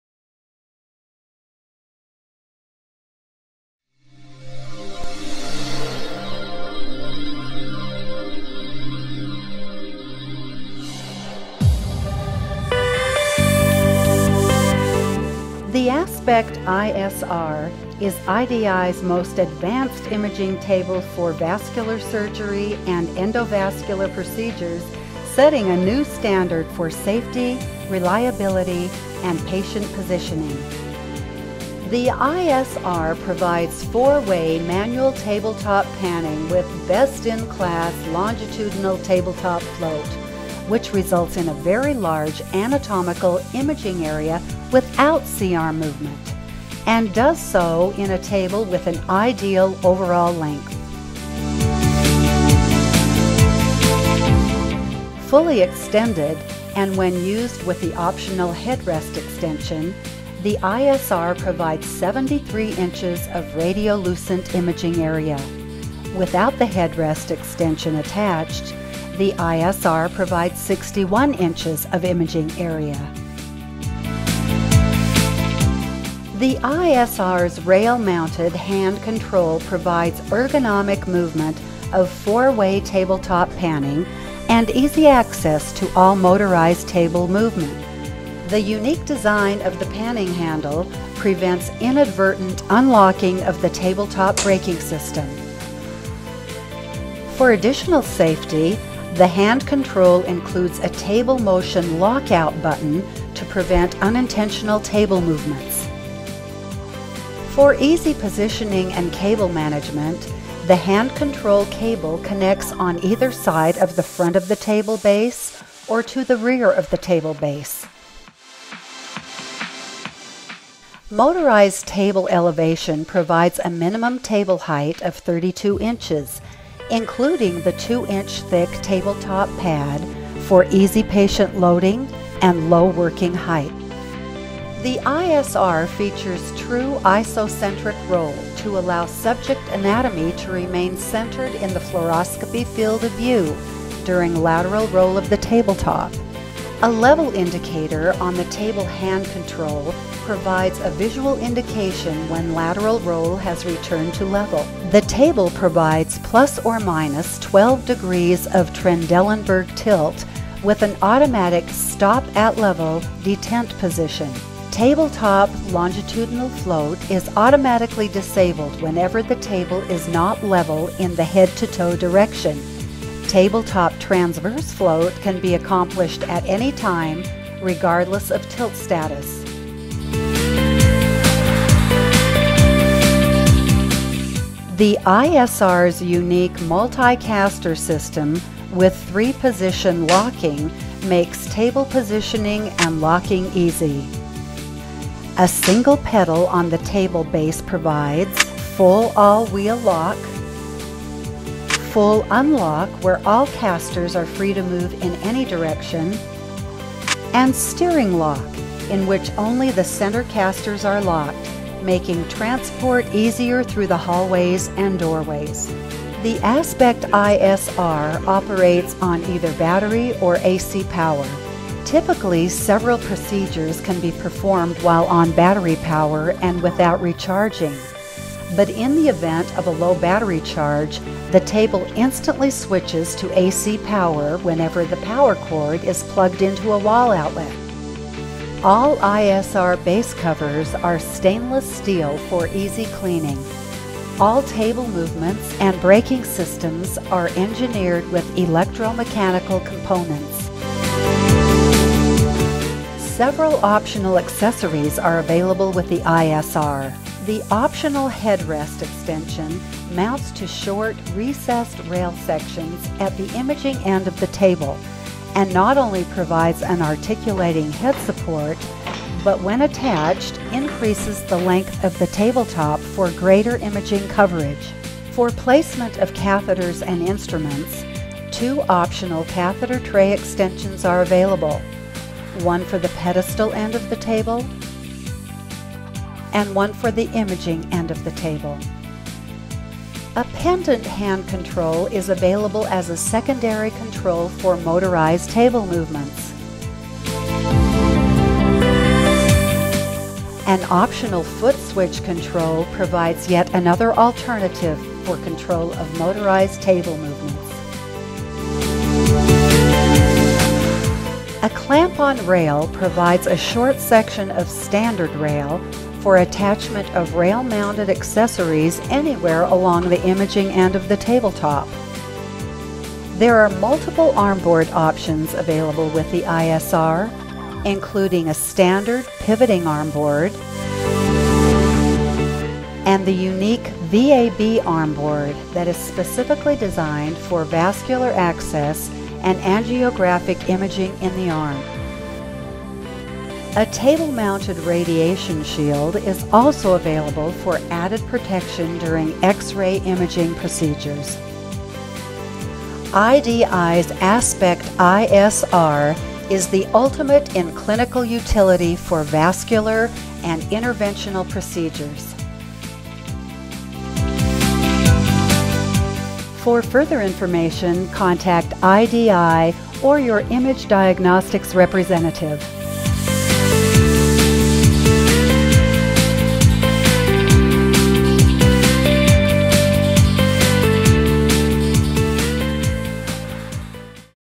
IDI ISR 4 way float table
Aspect ISR Table
The ultimate table for vascular & interventional procedures
Our most advanced imaging table delivers extended positioning and imaging capabilities:
• 4-Way Floating Tabletop
• Isocentric Lateral Tilt ("Iso-Roll") (Patent Pending)
• Trendelenburg Tilt ( ±12°)
• Motorized Elevation
Best-in-class longitudinal tabletop float of 32 inches for very large anatomical imaging coverage without C-arm movement Ideal overall length of 84 inches.
Tabletop Specifications:
Cantilevered, carbon fiber tabletop with low x-ray attenuation
Available with choice of tabletops:
  •  X100-2308 with side rails at pedestal       end of the table and 5" recessed       side-rail sections at the head end of       table
  •  X100-2337 includes side rails at
      pedestal end of table
Imaging area on main table body: 61" (157.5 cm)
Table movements:

Motorized Elevation: 32"- 40" with table pad
Trendelenburg Tilt: ±12° with "stop-at-level" positioning
4-way manual floating tabletop with panning hand control
Longitudinal tabletop travel: 32" (81.3 cm
Transverse tabletop travel: 8" (20.3 cm)
Multi-caster/3 position multi-lock system: total lock, total unlock and steering lock through center casters
Weight:

Patient weight capacity: 450 lbs. (204 kg)
Table weight: 605 lbs. (274 kg)
Electrical:

120 VAC or 230 VAC operation
(must specify on order)
Battery back-up operation
Accessories included:

Tabletop Pad
Patient Restraint Straps